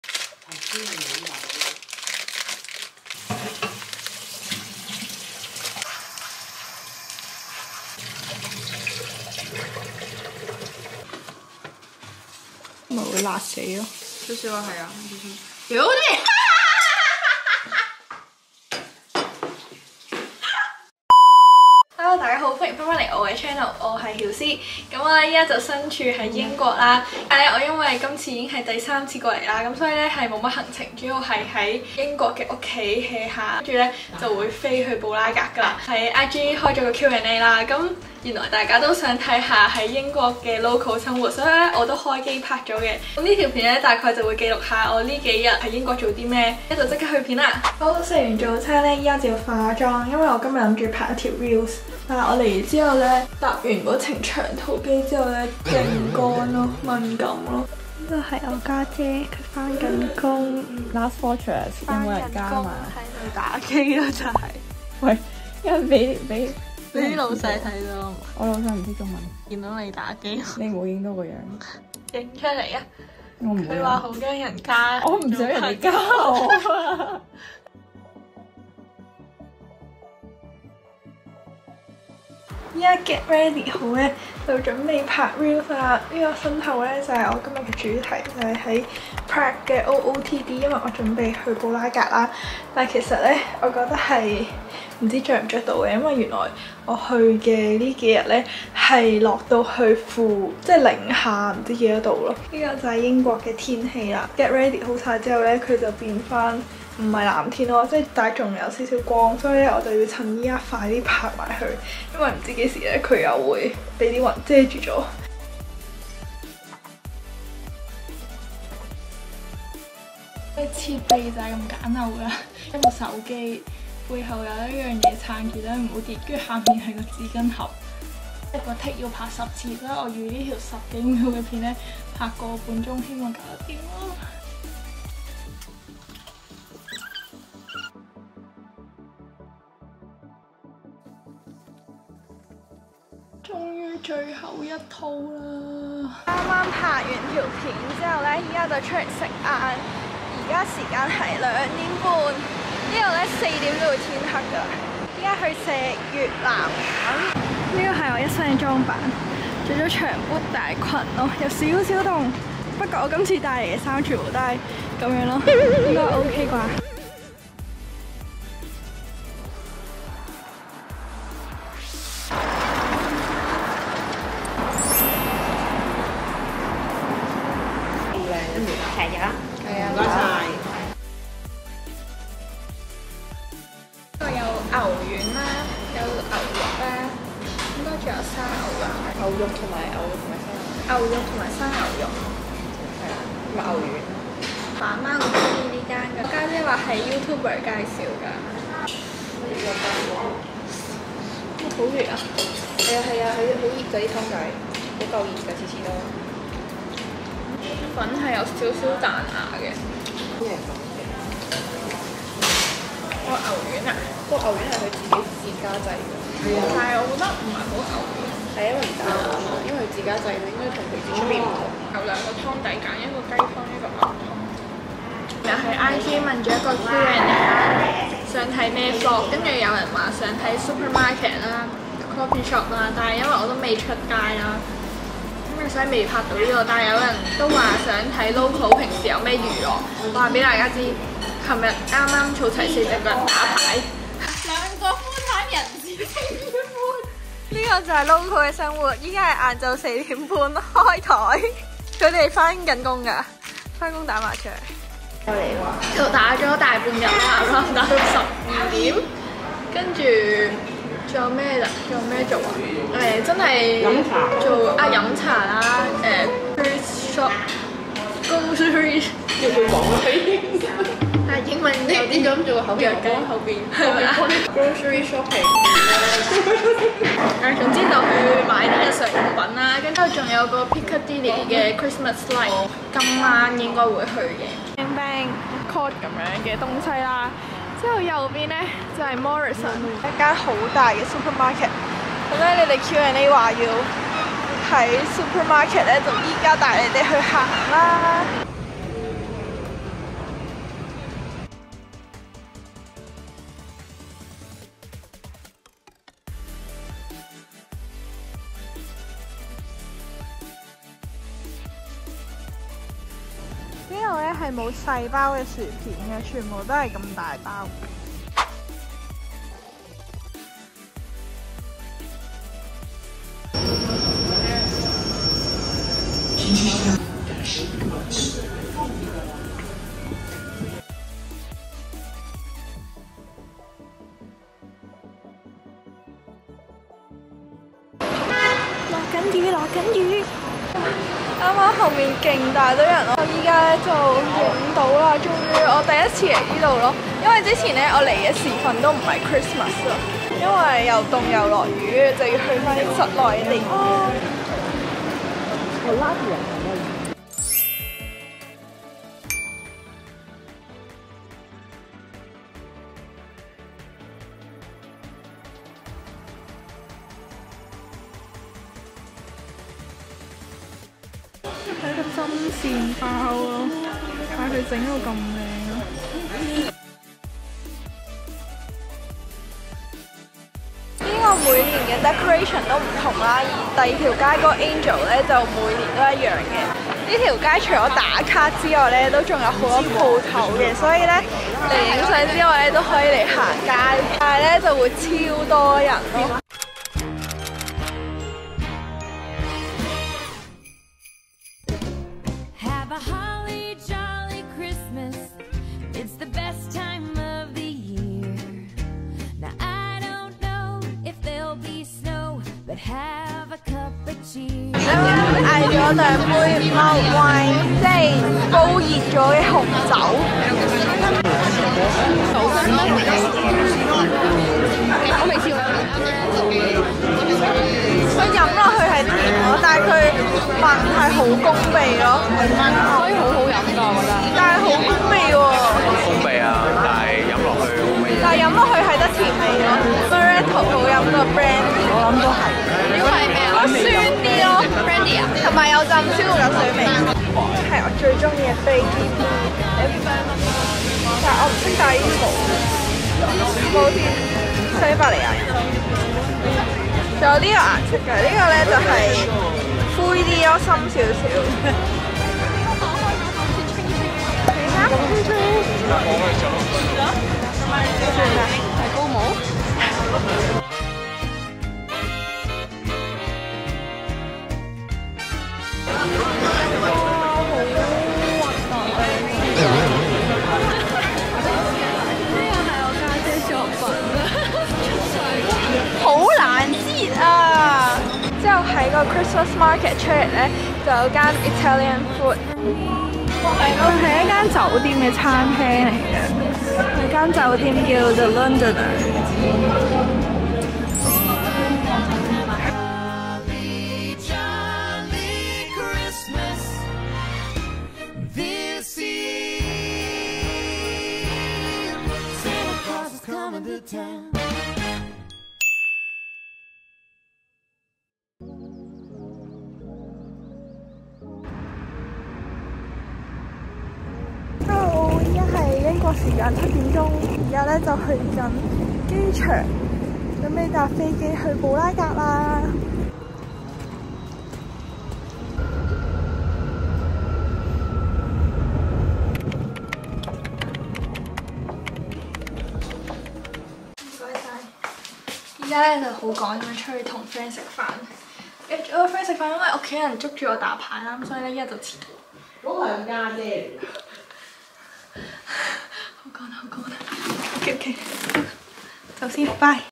【London Vlog🇬🇧23】解構移英港人生活｜真的如別人所說 超級悶？😮‍💨｜意外發現的有趣精品店｜著名聖誕燈飾 Piccadilly Lights👼🏻｜Christmas Market 🎄🎅🏻
2023年尾去了英國探訪姐姐
一起看看英國local的生活吧！
＊this video is edited by hiusze＊